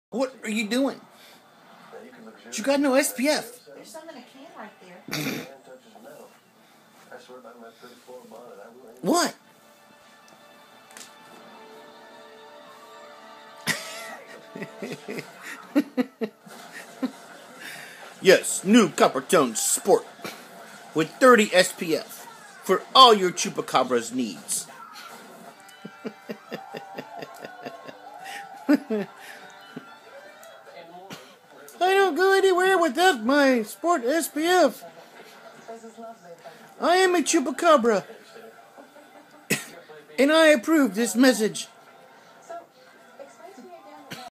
Chupacabras should use Coppertone Sunscreen!!!!!
Chupacabras should use Coppertone Sunscreen! I have adopted a Sphinx kitten and the monster is probably one of the strangest kitties I've owned.